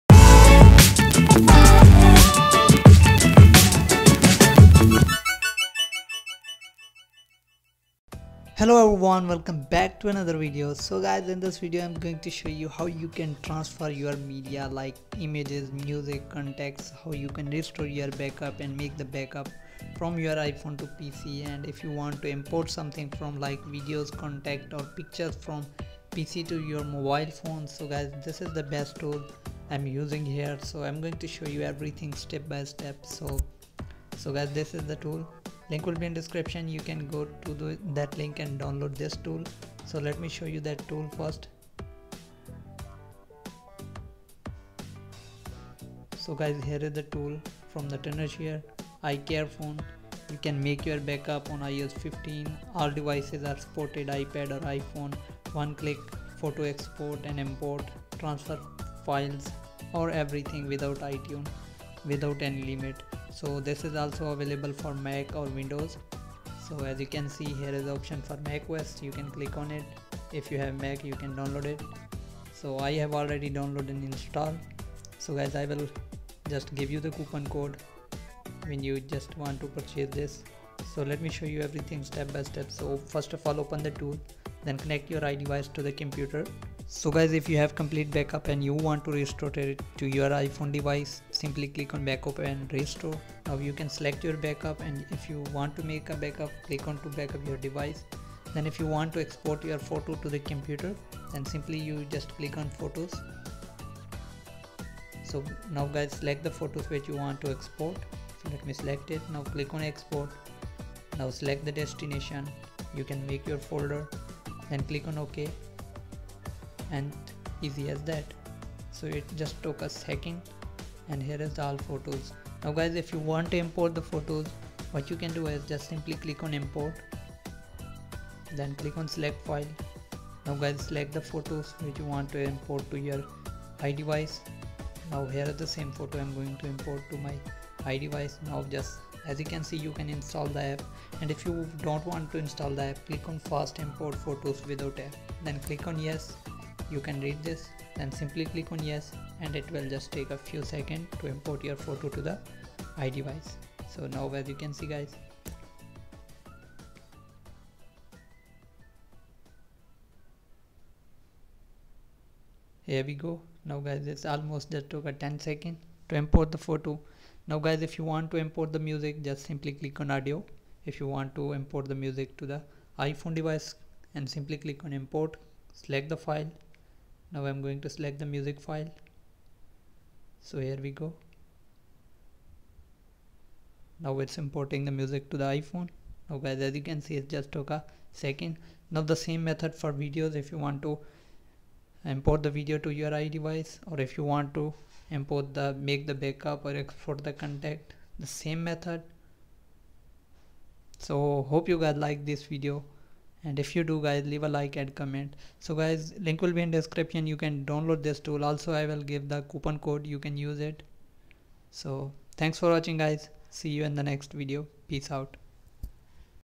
How to Transfer Photos From iPhone to Computer 2021 NewTenorshare iCareFone
Tenorshare iCareFone can flexibly manage your Contacts, Messages, Photos, Videos, Apps, Book, Bookmarks.